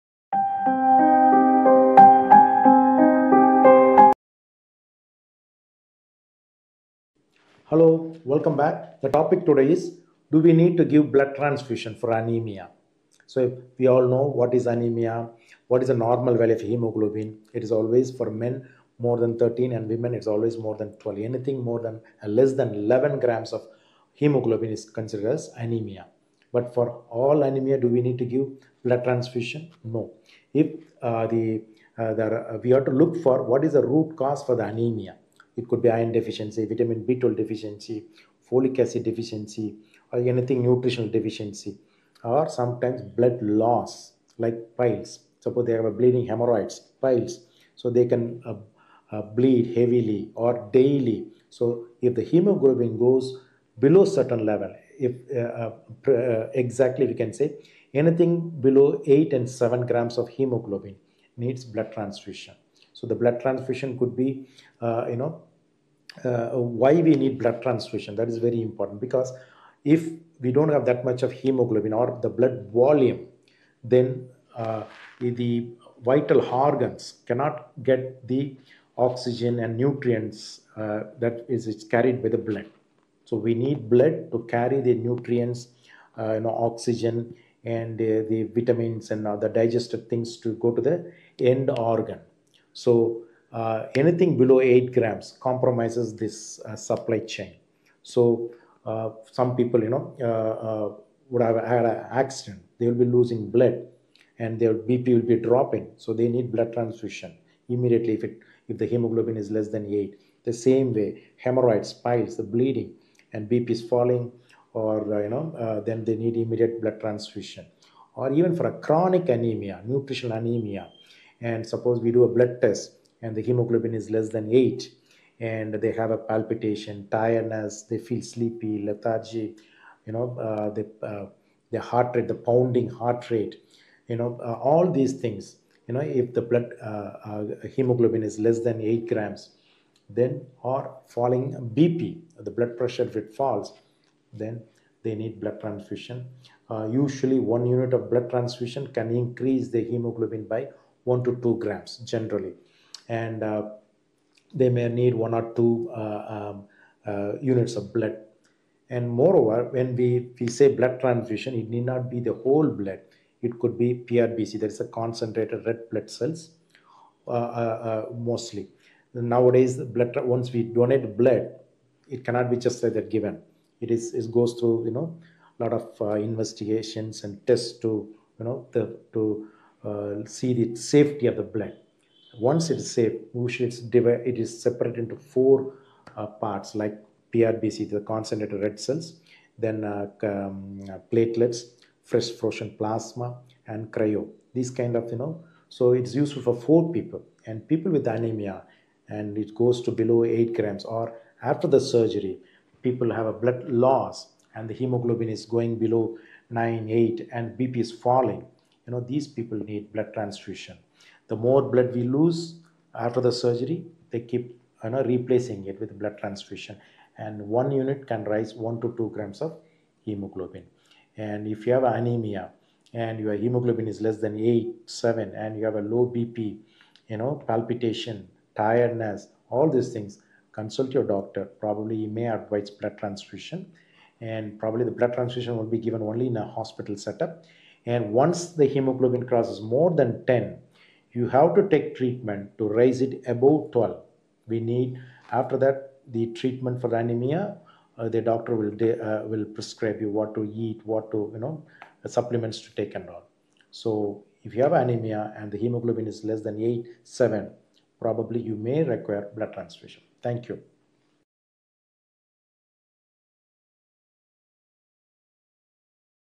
Anemia and Blood Transfusions | When is Blood Transfusion necessary if you have Anemia
In this video, Dr. Maran discusses when anemia requires blood transfusions. He starts by explaining that hemoglobin levels should be above 13 for men and 12 for women, with anything below 11 considered anemic. Not all anemic patients need a blood transfusion, as the cause—whether it’s due to nutritional deficiencies or blood loss—must be assessed first. Hemoglobin levels below 8 or 7 usually warrant a blood transfusion because organs aren't getting enough oxygen and nutrients, which can compromise their function.

Blood transfusions may also be required after surgery or in cases of severe trauma, piles, or other conditions causing blood loss. A falling blood pressure is another sign that a transfusion might be necessary. Typically, one unit of blood increases hemoglobin by 1-2 grams. Dr. Maran advises consulting a doctor and continuing treatment until hemoglobin levels reach at least 12.